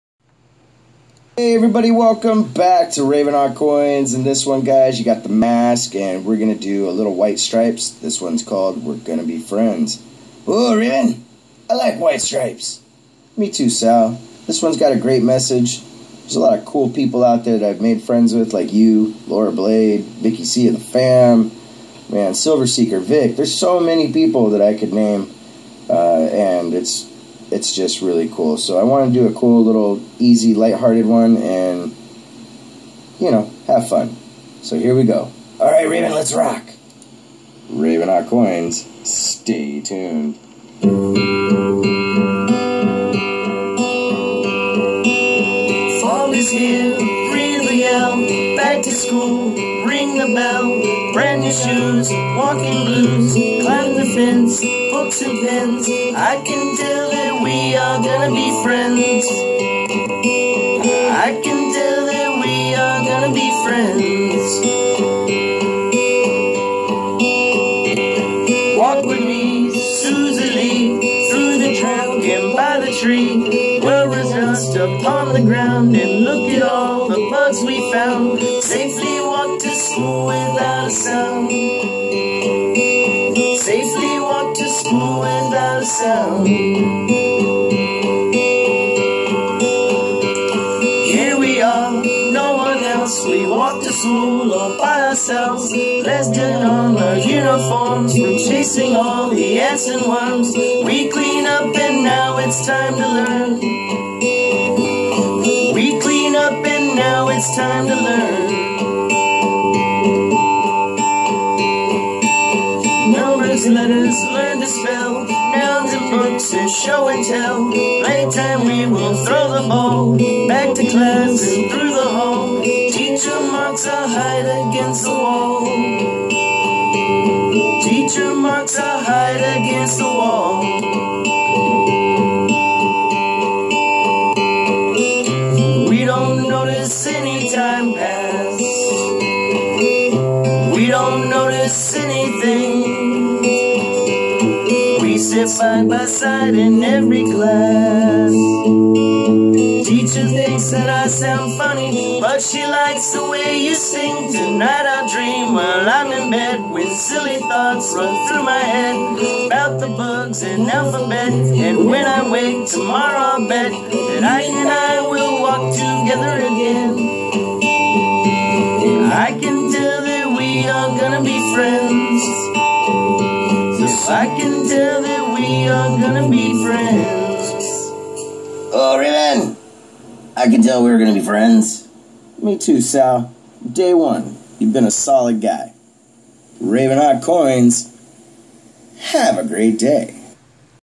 A lil fun for the group #70 White Stripes RavenHawk coins sings one for the community 😎😎
This time all alone.  Karaoke style again!!! Hope you enjoy and happy 🦃 day!! This is for the Coin Collecting, Stacking, Metal Detecting and Poured bars group! SAL as the Djay here is a link to his channel